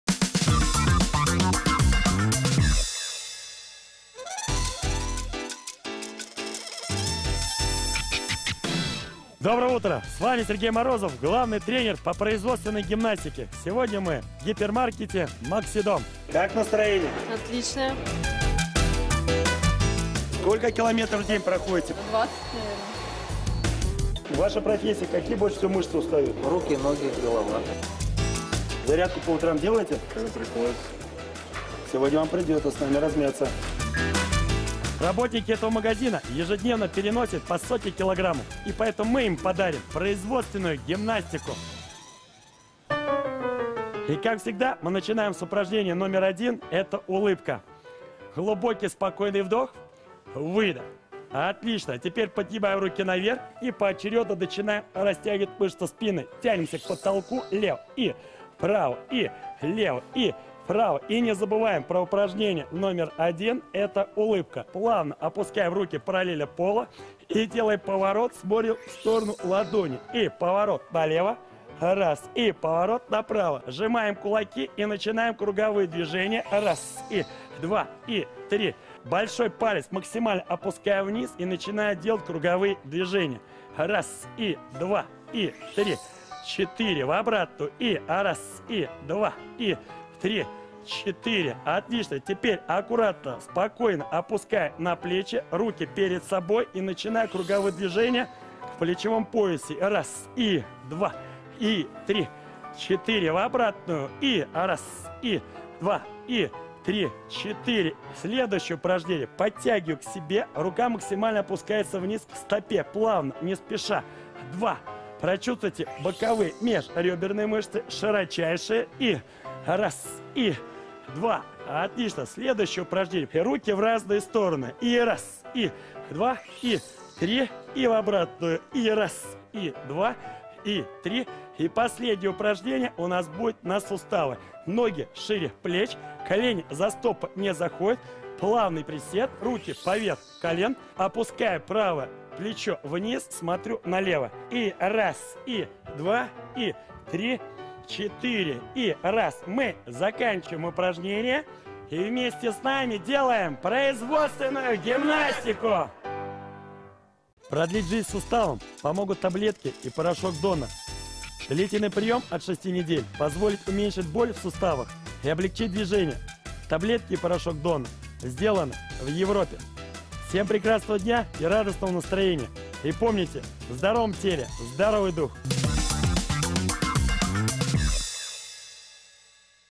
Физкультминутка в гипермаркете
Работники большого магазина ежедневно переносят по сотне килограммов, поэтому главный тренер сборной по производственной гимнастике Сергей Морозов подобрал им для физкультминутки упражнения, направленные на растяжку мышц спины, рук и ног.